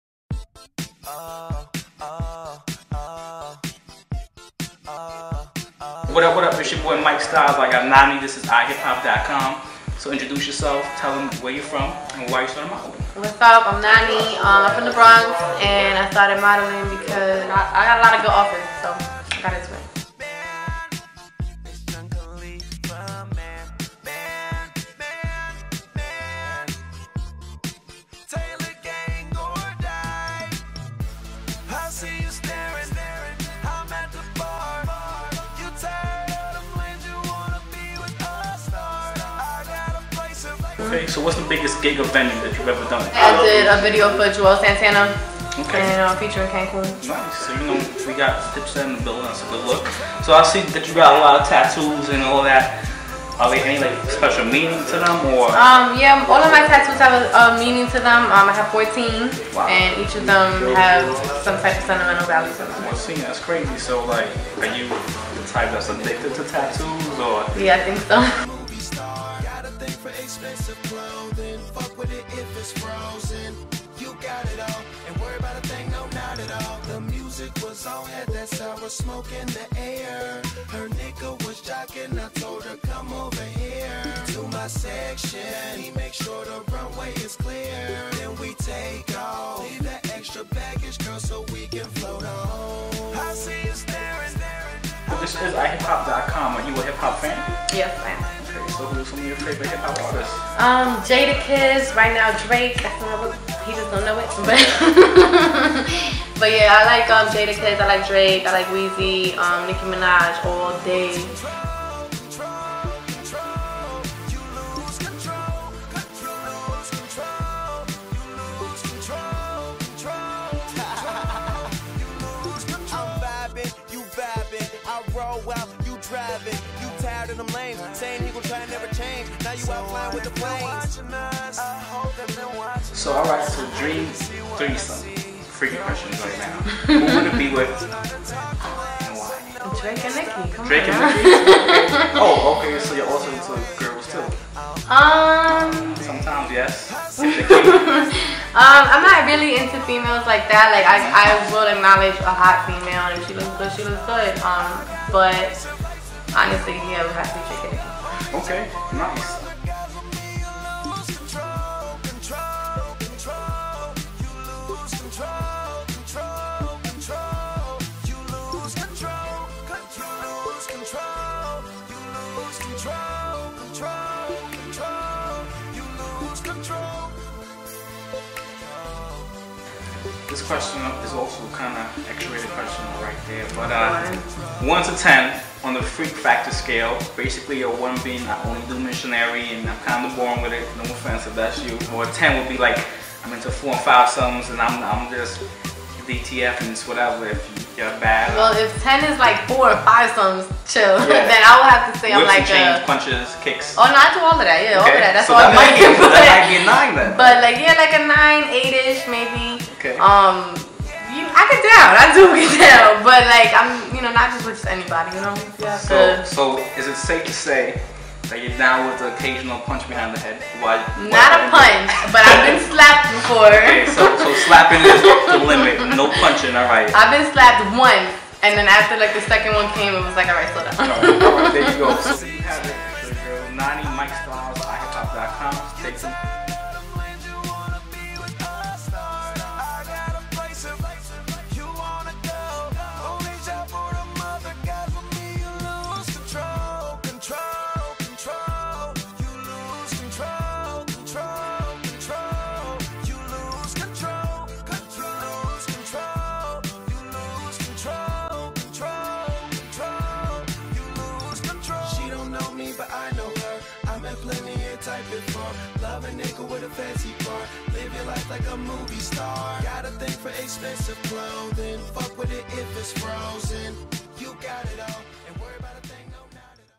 iHipHop Model Chick: Nani (Music By Wiz Khalifa)
iHipHop model chick Nani shows off her dancing moves and gets interviewed by iHipHop blogger Mike Stylezz.
Visit iHipHop.com for more Model Chicks